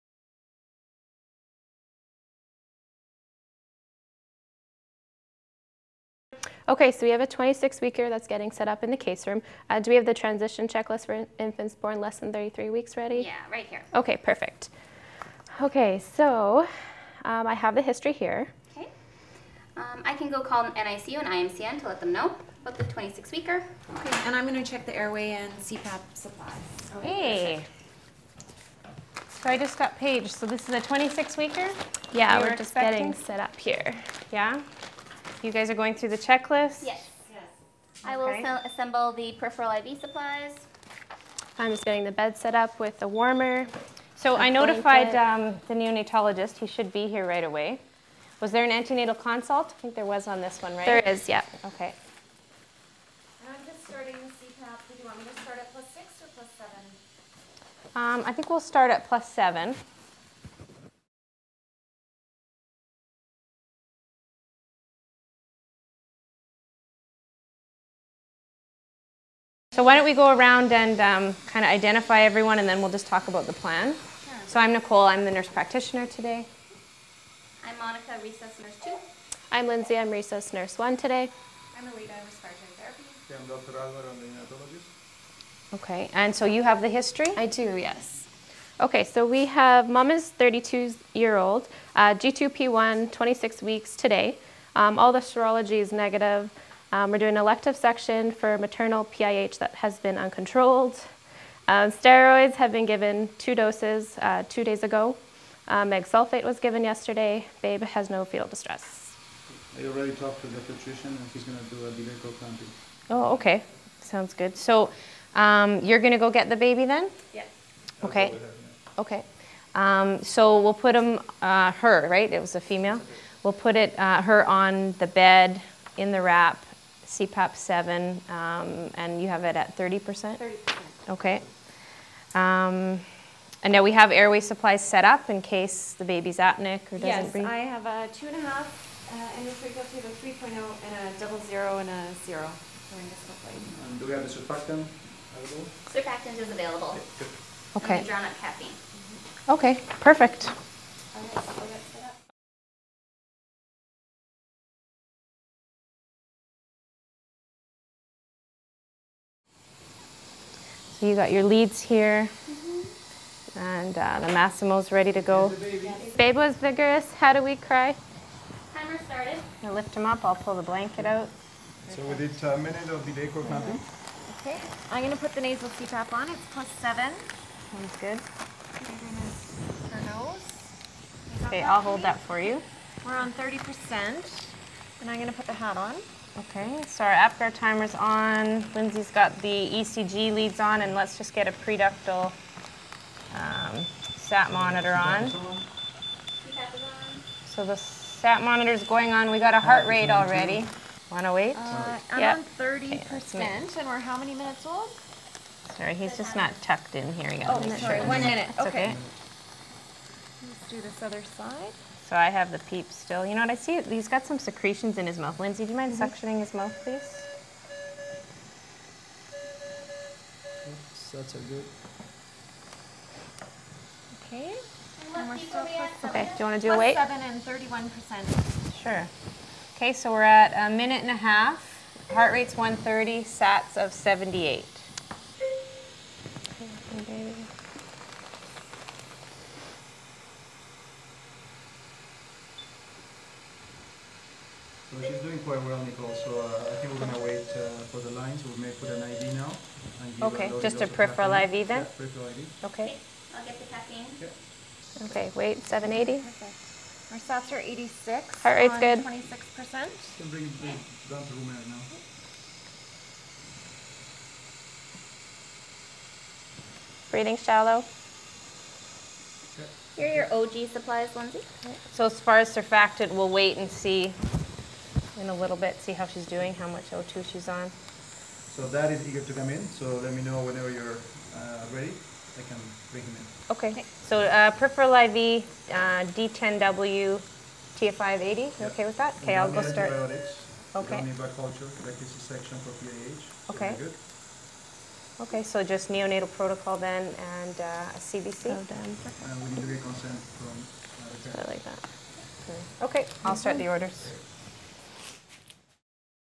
What Is The Golden Hour Approach?
Preparing for the delivery of a premature infant who will require CPAP.  The “Golden Hour” of  is defined as the 1st hour of post-natal life in both preterm and term neonates. The approach is based on the notion that teams will practice evidence based intervention in the initial sixty minutes of postnatal life for better long-term outcome.